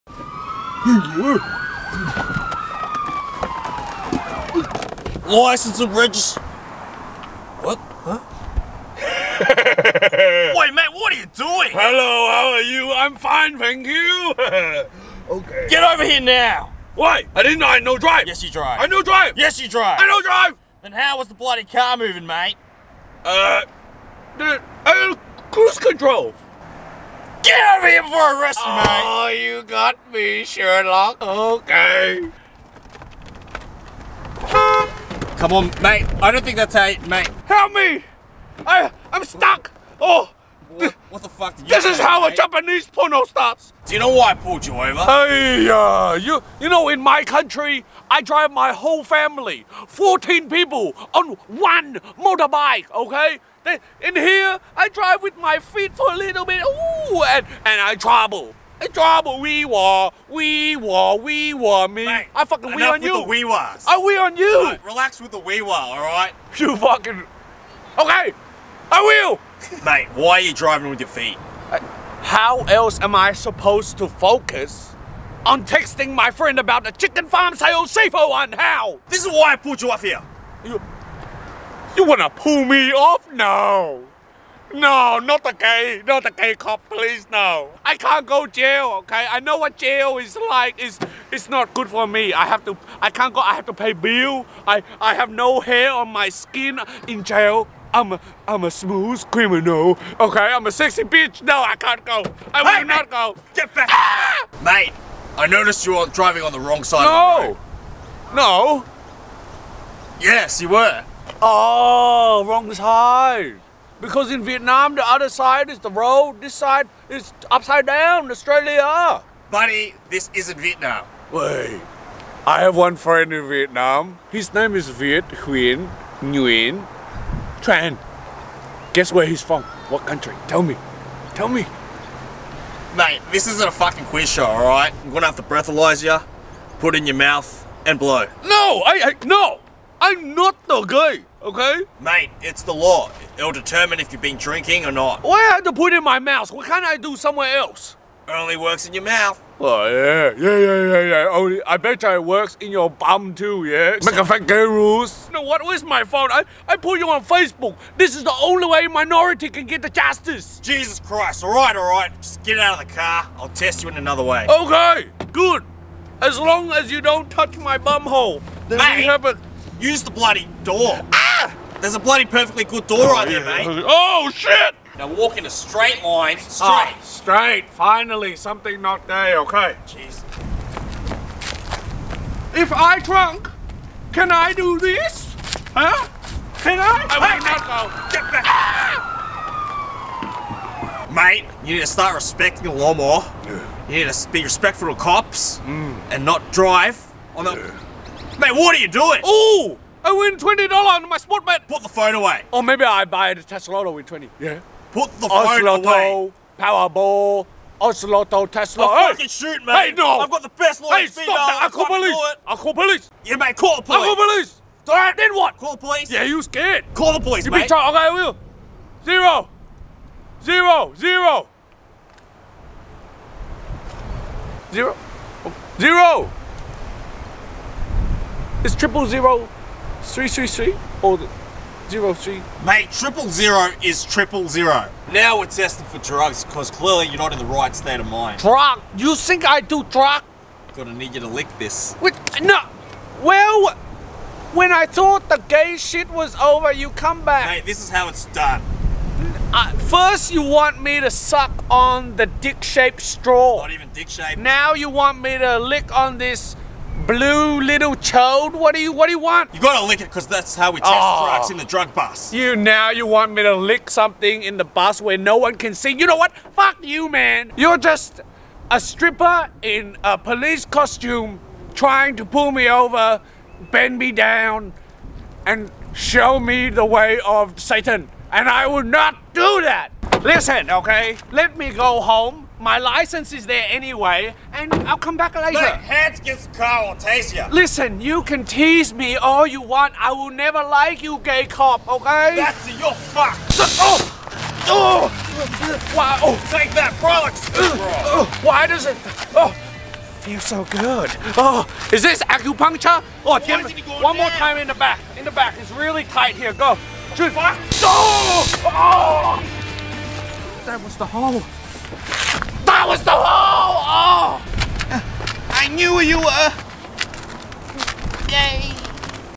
Asian dad gets pulled over by COPS
HOW ARE YOU? I'M FIVE THANK YOU!

Fun fact: I gained weight for this role + took me 9 months to grow that moustache

Big shout out to Tom for helping me film @tomvu91 IG